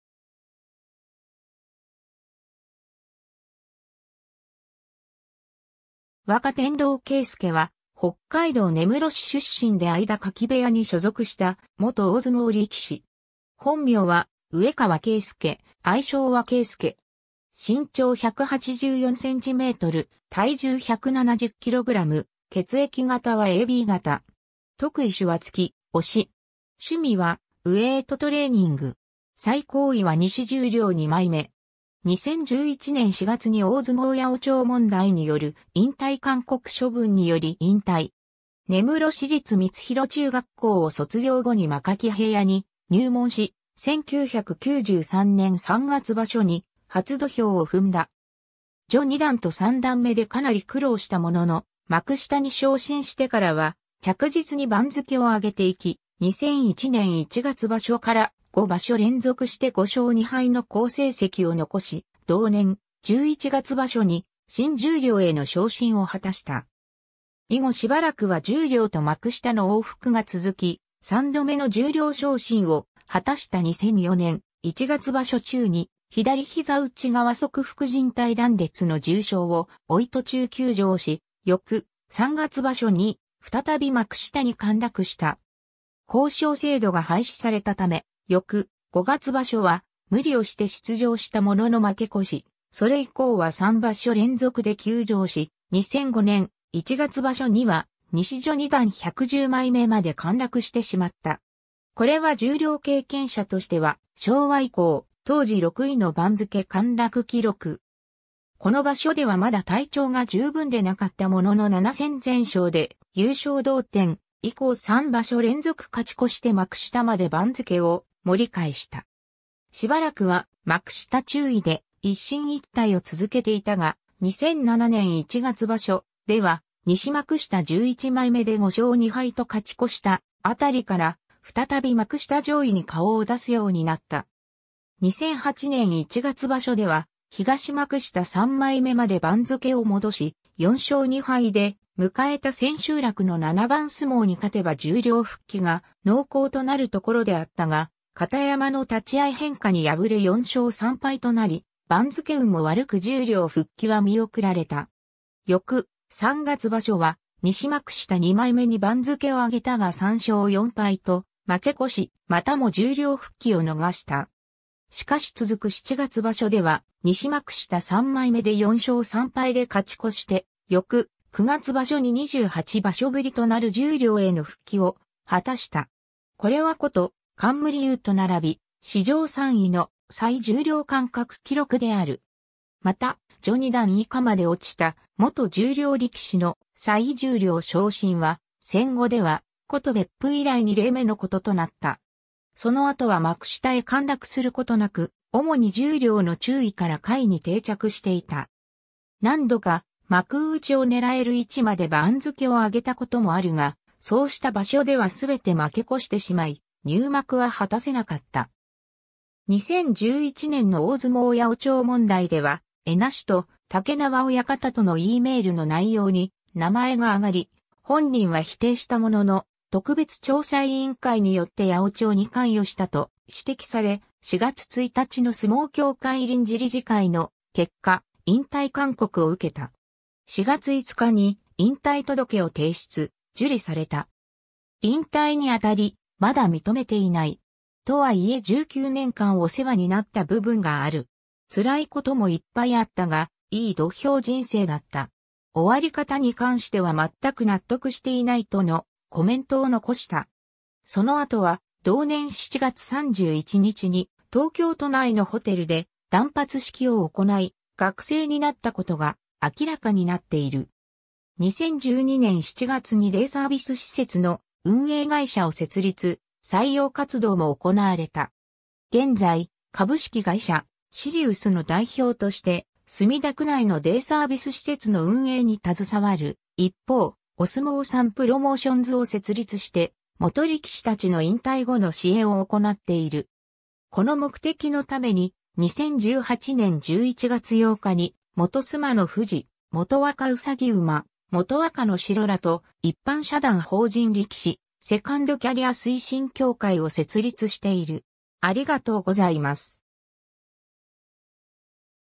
若天狼啓介
若天狼 啓介（わかてんろう けいすけ、1977年10月18日 - ）は、北海道根室市出身で間垣部屋に所属した元大相撲力士。
本名は上河 啓介（かみかわ けいすけ）、愛称はケイスケ。
身長184cm、体重170kg、血液型はAB型。
得意手は突き、押し。
趣味はウエイトトレーニング。
最高位は西十両2枚目（2010年9月場所）。
2011年4月に大相撲八百長問題による引退勧告処分により引退。
 根室市立光洋中学校を卒業後に間垣部屋に入門し、1993年3月場所に初土俵を踏んだ。
序二段と三段目でかなり苦労したものの、幕下に昇進してからは着実に番付を上げて行き、2001年1月場所から5場所連続して5勝2敗の好成績を残し、同年11月場所に新十両への昇進を果たした。
以後しばらくは十両と幕下の往復が続き、3度目の十両昇進を果たした2004年1月場所中に左膝内側側副靱帯断裂の重傷を負い途中休場し、翌3月場所に再び幕下に陥落した。
 公傷制度が廃止されたため、翌5月場所は無理をして出場したものの負け越し、それ以降は3場所連続で休場し、2005年1月場所には西序二段110枚目まで陥落してしまった。
これは十両経験者としては昭和以降、当時6位の番付陥落記録。
この場所ではまだ体調が十分でなかったものの7戦全勝で優勝同点、以降3場所連続勝ち越して幕下まで番付を盛り返した。
しばらくは幕下中位で一進一退を続けていたが、2007年1月場所では西幕下11枚目で5勝2敗と勝ち越したあたりから、再び幕下上位に顔を出すようになった。
 2008年1月場所では東幕下3枚目まで番付を戻し、4勝2敗で迎えた千秋楽の7番相撲に勝てば十両復帰が濃厚となるところであったが、片山の立合い変化に敗れ4勝3敗となり、番付運も悪く十両復帰は見送られた。
翌3月場所は西幕下2枚目に番付を上げたが3勝4敗と負け越し、またも十両復帰を逃した。
しかし続く7月場所では西幕下3枚目で4勝3敗で勝ち越して、翌9月場所に28場所ぶりとなる十両への復帰を果たした。
これは琴冠佑と並び、史上3位の再十両間隔記録である。
また、序二段以下まで落ちた元十両力士の再十両昇進は、戦後では琴別府以来2例目のこととなった。
その後は幕下へ陥落することなく、主に十両の中位から下位に定着していた。
何度か幕内を狙える位置まで番付を上げたこともあるが、そうした場所では全て負け越してしまい、入幕は果たせなかった。
 2011年の大相撲八百長問題では恵那司と竹縄親方とのEメールの内容に名前が挙がり、本人は否定したものの、特別調査委員会によって八百長に関与したと指摘され、4月1日の相撲協会臨時理事会の結果、引退勧告を受けた。
4月5日に引退届けを提出、受理された。
引退にあたり「まだ認めていない。とはいえ19年間お世話になった部分がある。辛いこともいっぱいあったが、いい土俵人生だった。終わり方に関しては全く納得していない」とのコメントを残した。
その後は同年7月31日に東京都内のホテルで断髪式を行い、学生になったことが明らかになっている。
2012年7月にデイサービス施設の運営会社を設立、採用活動も行われた。
 現在、株式会社シリウスの代表として墨田区内のデイサービス施設の運営に携わる一方、お相撲さんプロモーションズを設立して元力士たちの引退後の支援（体の大きな人材、相撲のネット中継の解説者のキャスティングなど）を行っている。
この目的のために、2018年11月8日に元須磨ノ富士・元若兎馬、元若ノ城らと一般社団法人力士セカンドキャリア推進協会を設立している。